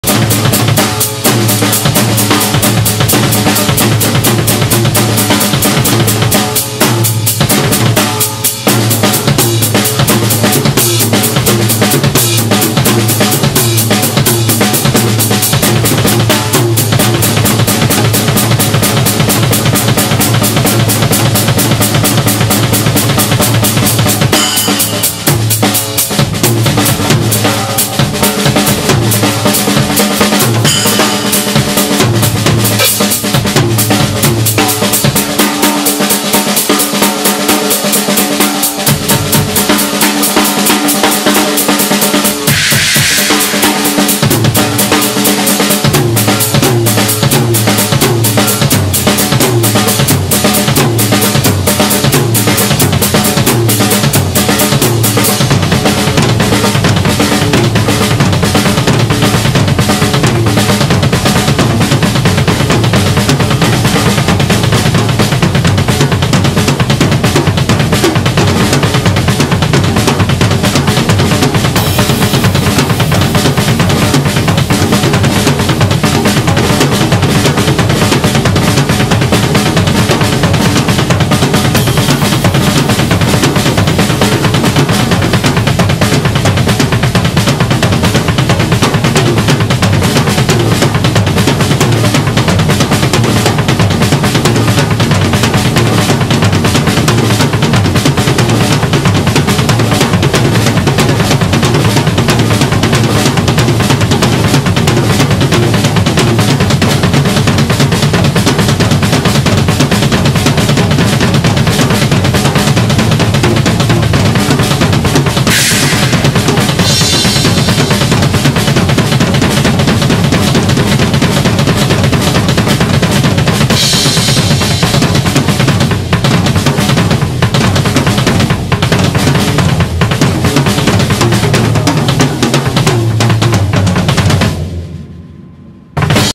Ludwig 7ply Maple Classic Kit with Paiste, Zildjian & Sabian Cymbals. Headphones Recommended 🎧
Recorded with iPhone12 Pro Max and Shure MV88 +